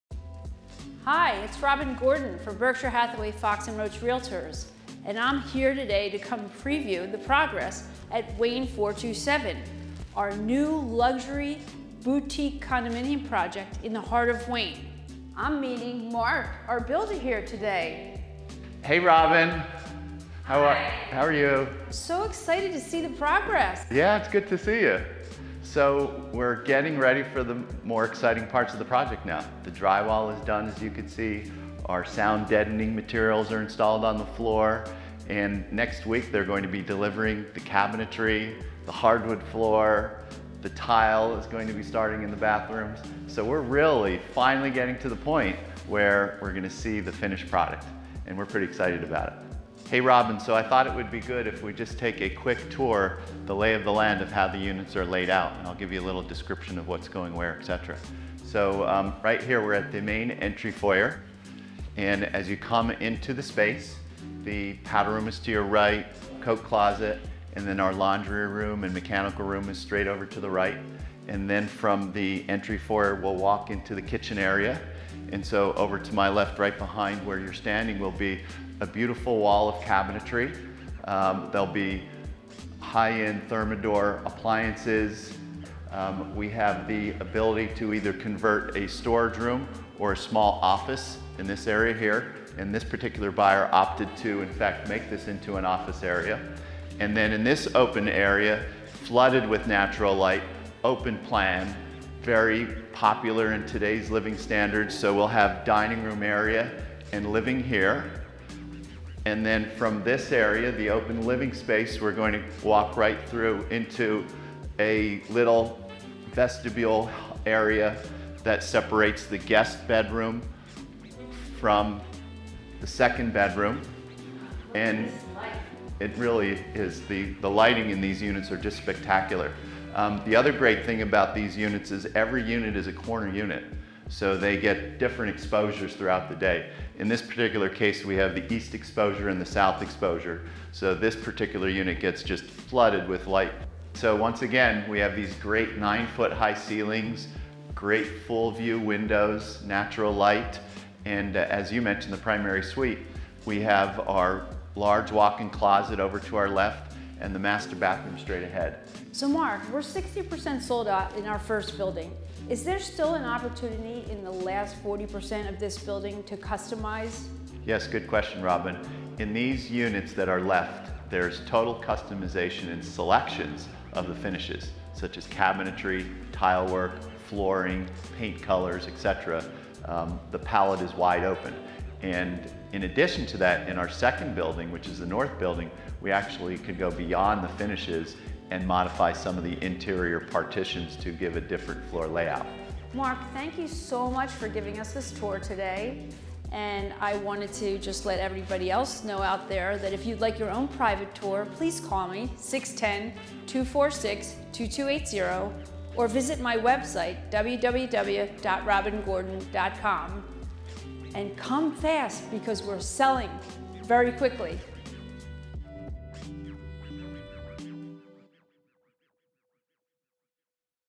Wayne 427 Tour feb2021
Wayne 427 is a new luxury residential condominium in a green, established Main Line setting, built by Janiczek Homes. There are 2 modern buildings with 11 homes in each, thoughtfully planned with only 4 units per floor. Each home occupies a corner position, optimizing light and views through the floor to ceiling windows. 2 and 3 bedroom units, and penthouse suites are available. Just a few of the special features include a green roof garden, pet-friendly park, elevators, keyless entry, underground parking garage, individual storage, gym, and a mail/package room. Landscaping and snow removal provides you a low maintenance lifestyle! Walk to Wayne restaurants and shops, or hop the Wayne or St. Davids train to Center City! To learn about the builder, Janiczek Homes,